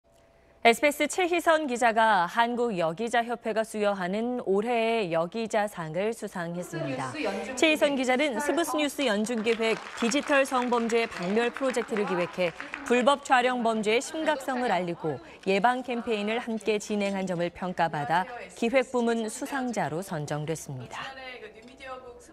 SBS 채희선 기자 '올해의 여기자상' 수상 / SBS
SBS 채희선 기자가 한국여기자협회가 수여하는 올해의 여기자상을 수상했습니다.
채희선 기자는 스브스뉴스 연중기획 '디지털 성범죄 박멸 프로젝트'를 기획해 불법 촬영 범죄의 심각성을 알리고 예방 캠페인을 함께 진행한 점을 평가받아 기획 부문 수상자로 선정됐습니다.